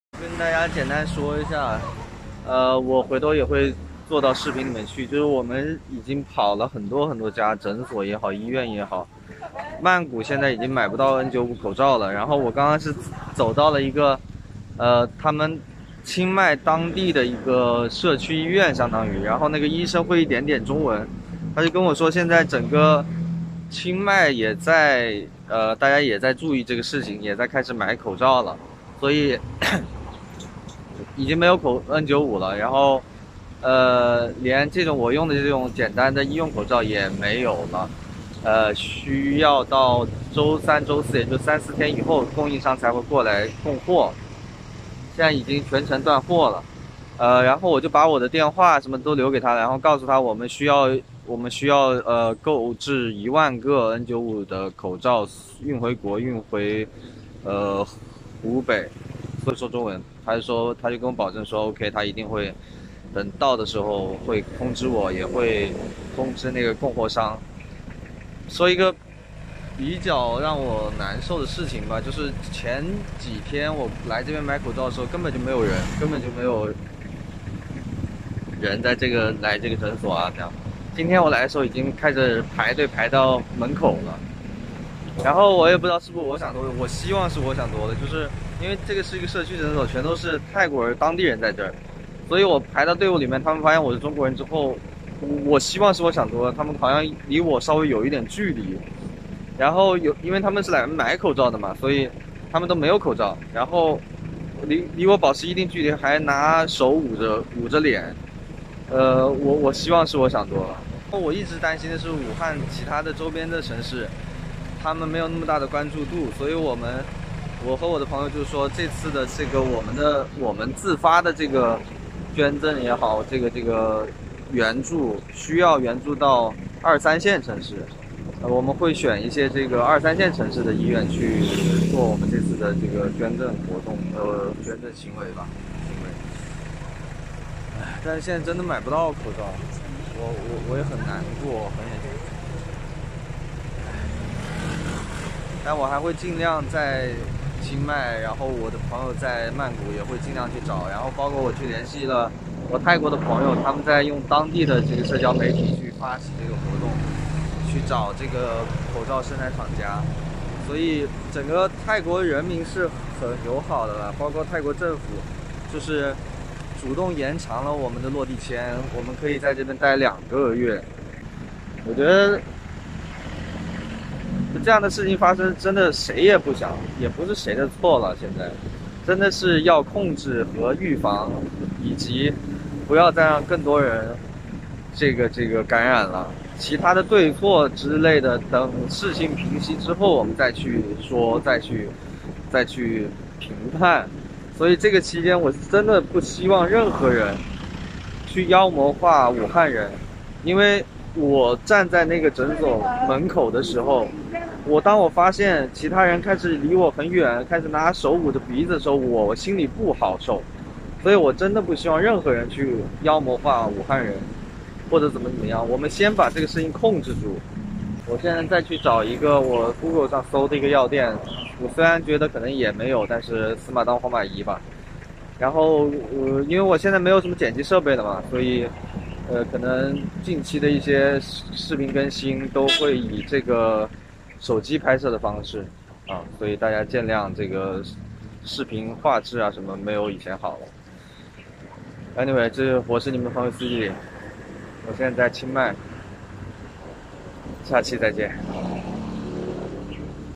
在清邁找口罩支援湖北！希望每個人都盡自己所能，一起戰勝病毒！Trying to buy mask to help Chinese people prevent the Corona-virus
之前發的動態我們在泰國找口罩郵寄回湖北，更新一下進度，不是很樂觀，但是我們幾個人都還在努力爭取！